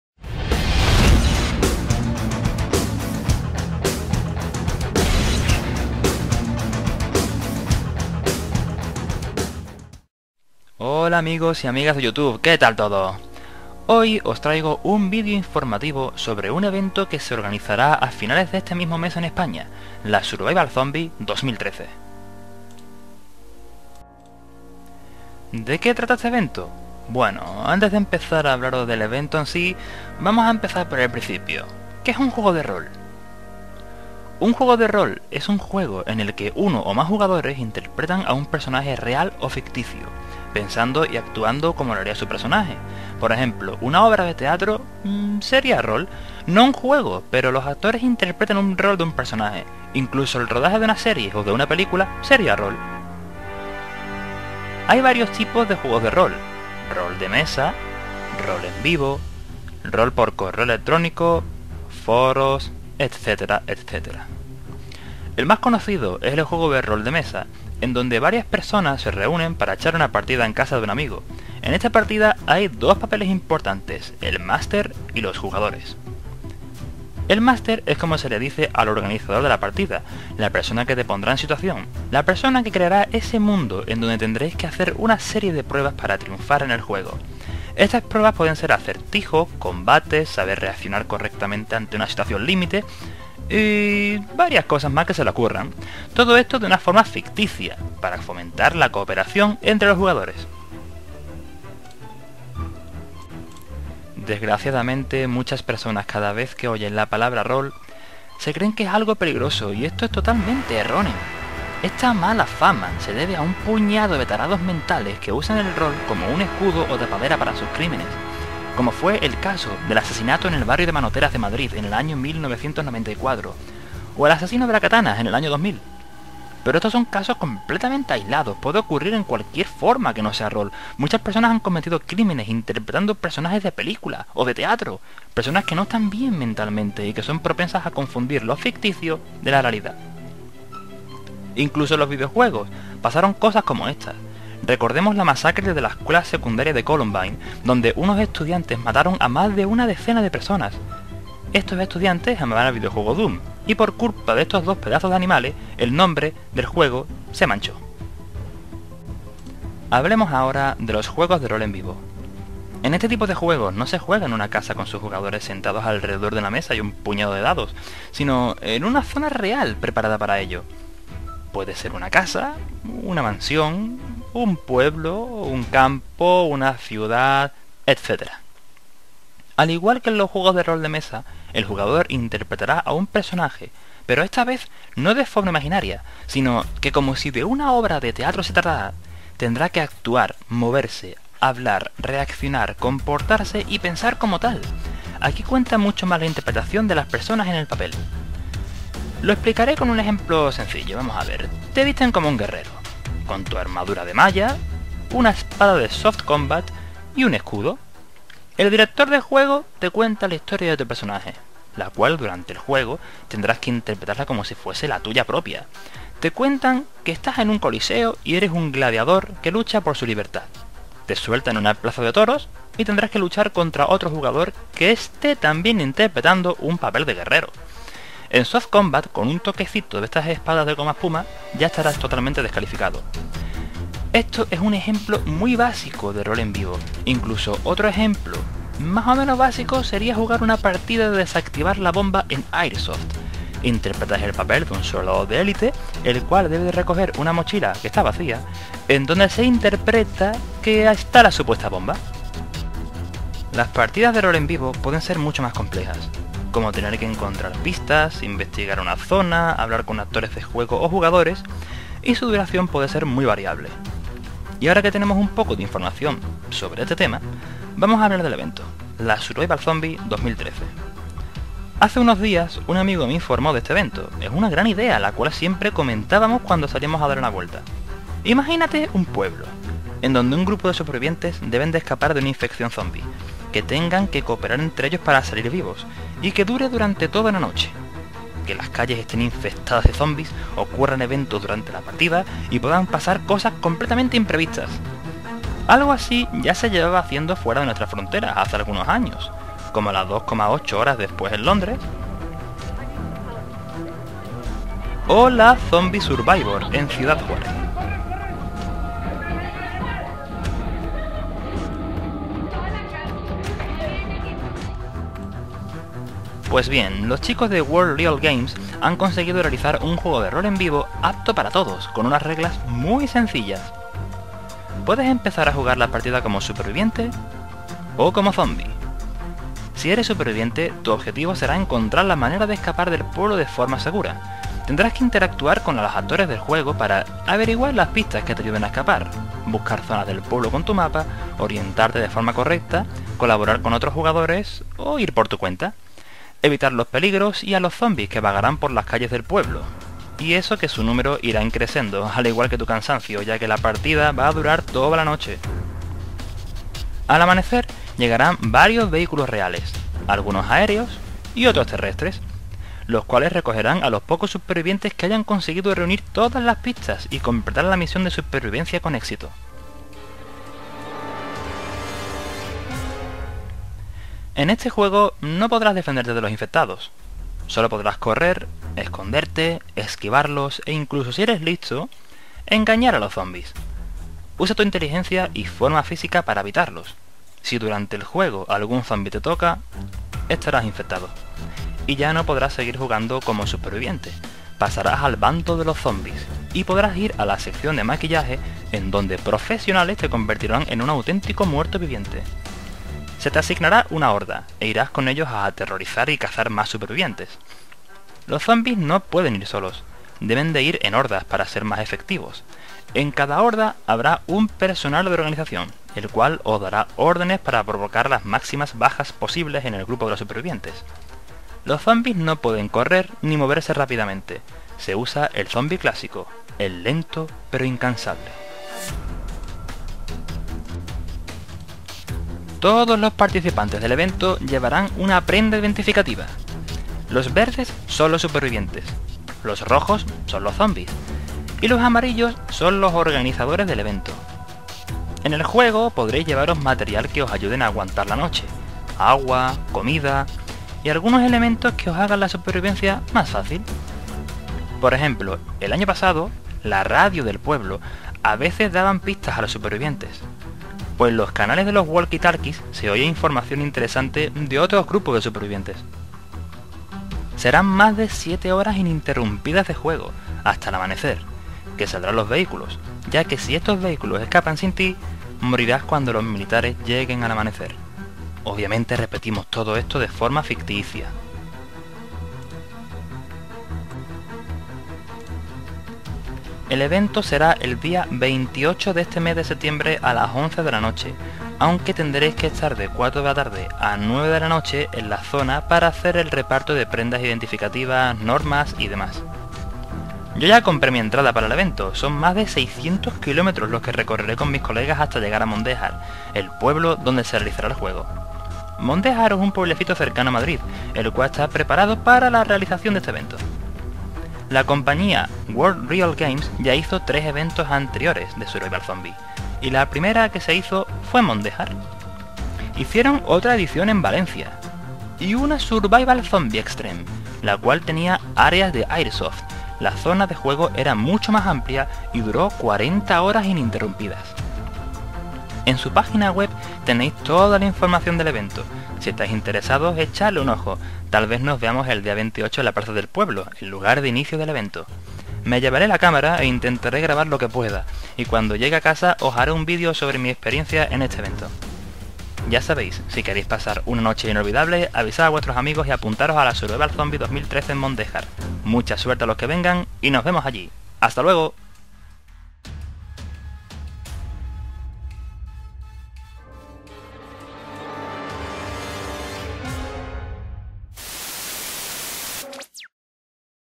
-Survival Zombie 2013- [¿Que significa ROL?]
Hablemos un poco sobre este nuevo evento de nuestro pais, y lo que significa rol, ya que la gente a veces confunde el rol como algo peligroso...